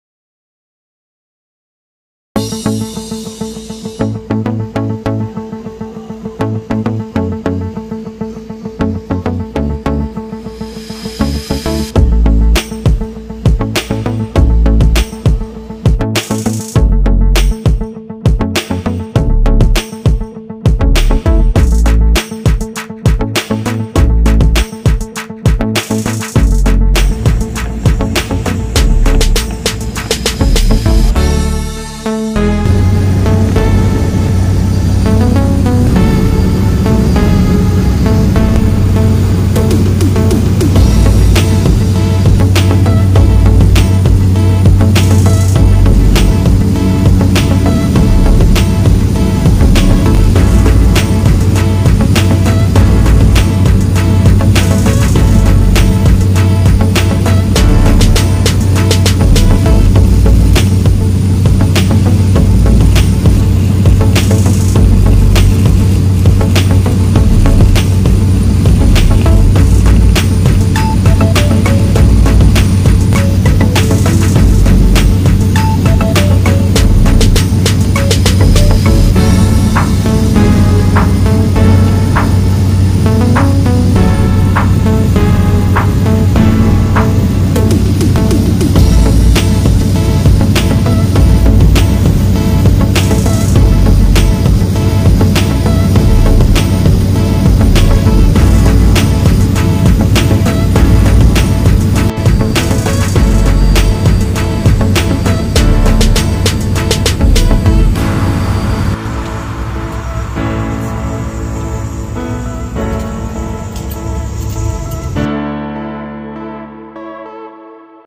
Honolulu to Lanai Scenic Plane Ride : Mokulele Airlines (Travel Guide)
a beatiful scenic plane ride from Honolulu Oahu to Lanai Island Hawaii via Mokolele Airlines . Hawaii Travel guide.